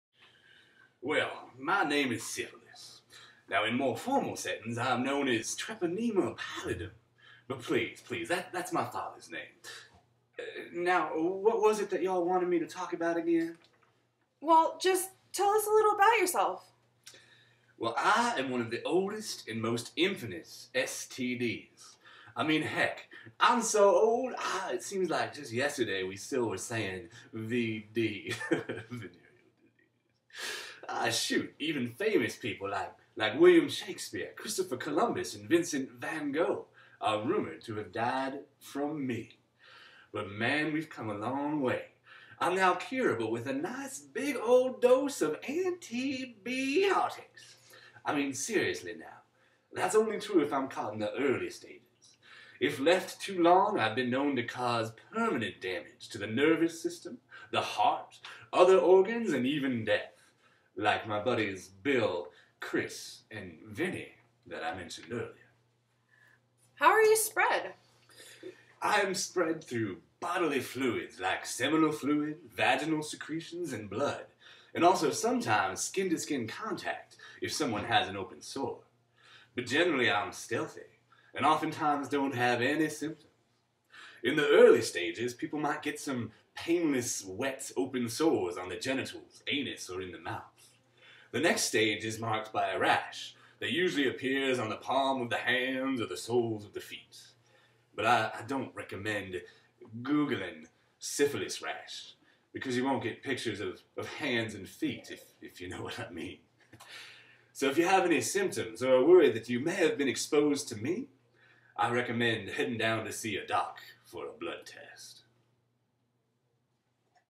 Confessions of an STD: Meet Syphilis
Syphilis: Well my name is Syphilis, in more formal settings I am
known as Treponema Pallidum, but please please that is my father's name. Uhh what was it that ya'll wanted me to talk about?

Announcer: Well just tell us a little about yourself.

Syphilis: I am one of the oldest and most infamous STDS. I mean,
heck, I am so old it seems like yesterday that we were still saying VD. (Snorts/Laughs)

Shoot even famous people like William Shakespeare, Christopher Columbus and Vincent Van Gogh are rumored to have died from me.

But, man we have come a long way. I am now curable with a nice big ol' dose of a-n-t-e-biotics. Seriously now, that's only true if I am caught in the early stages. If left to early too long I have been known to cause permanent damage to the nervous system, heart, other organs and even death, like my buddies Bill, Chris and Vinny that I mentioned before.

Announcer: How are you spread?

Syphilis: I am spread through bodily fluids like seminal fluid, vaginal secretions and blood, also some skin to skin contact if someone has an open sore.

But generally I am stealthy and often times don't have any symptoms. In the early stages people might get some painless-wet-open sores on the genitals anus or in the mouth.

The next stage is marked by a rash that usually appears on the palm of the hands or soles of the feet. I don't recommend googling syphilis rash because you won't get pictures of hands and feet, if you know what I mean.

So, if you have any symptoms or are worried that you may have been exposed to me I recommend heading down to see your doc for a blood test.